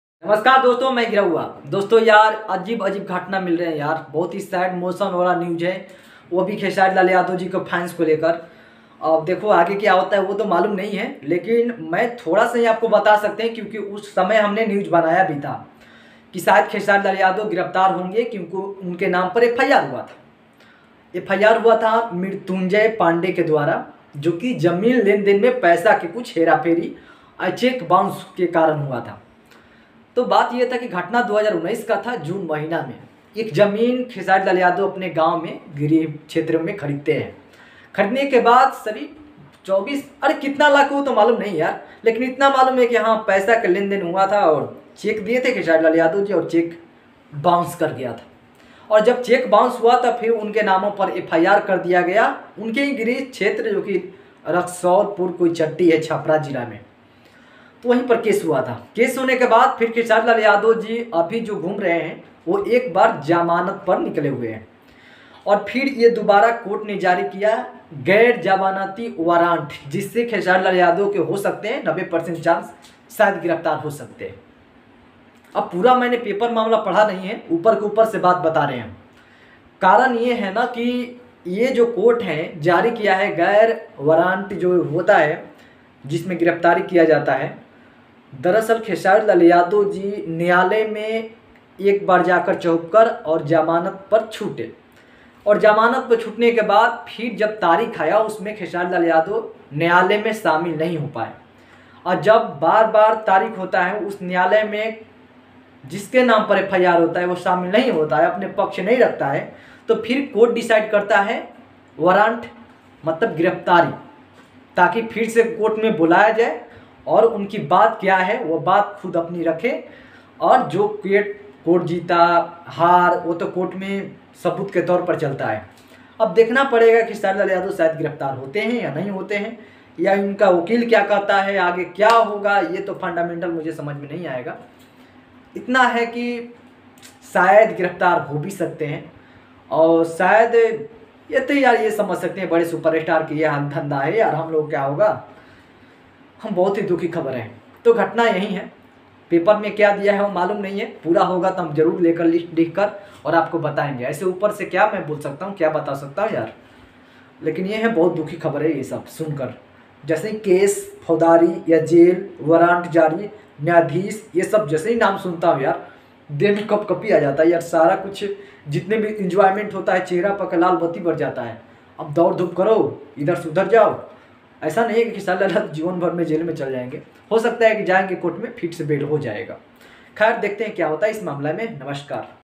kheshariगिरफतर होगे kheshari Lal yadav कोर्ट ने गैर जमानती वारंट | खेसारी लाल पर फोर्ड का मामला !!
khesarilalyadav ने होंगे गिरफ्तार कोट ने दी फासला
खेसारी लाल यादव ने फोर्ट h